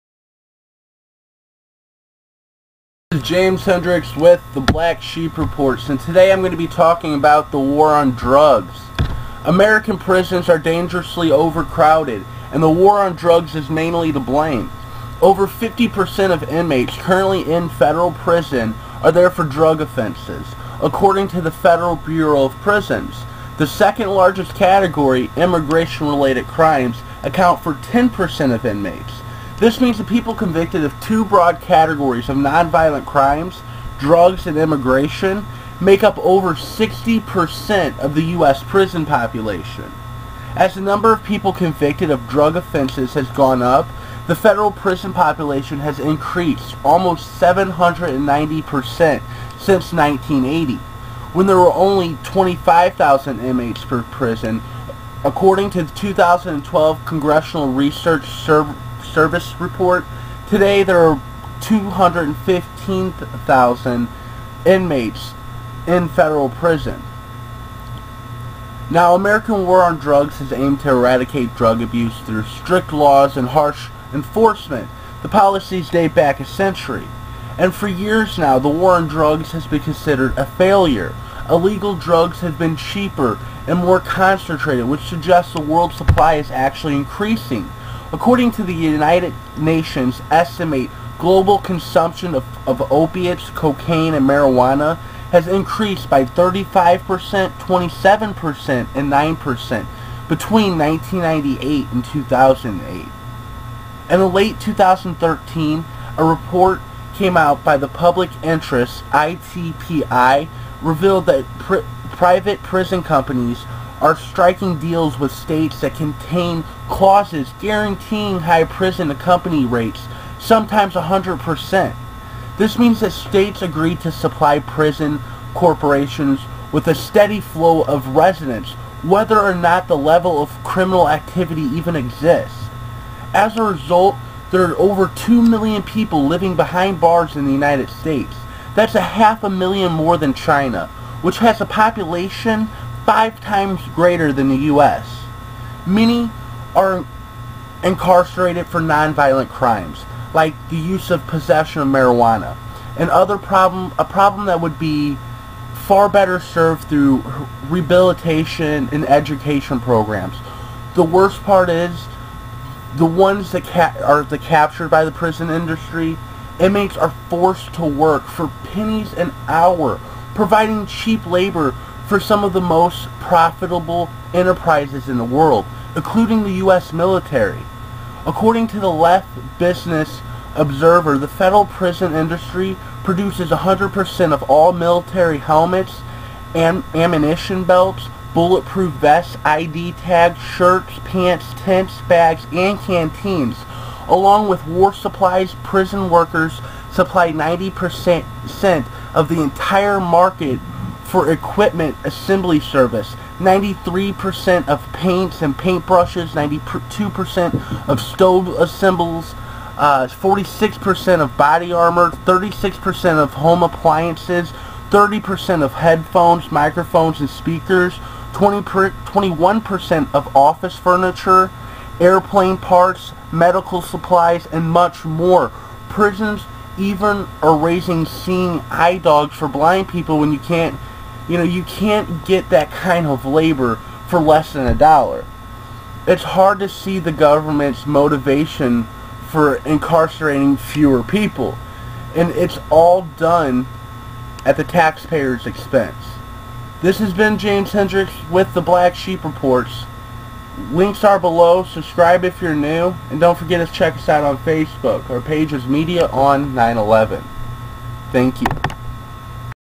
Black Sheep Reports:War On Drugs!
Today on the Black Sheep Reports James Hendricks talks about the War On Drugs and how drugs and immigration  make up over 60 percent of the U.S. prison population. He talk about how The Federal Prison Population has increased  almost 790 percent since 1980. Also he talks about Private prison companies are making a profit off of cheap labor paying imamates less than a dollar and much more today on the Black Sheep Reports.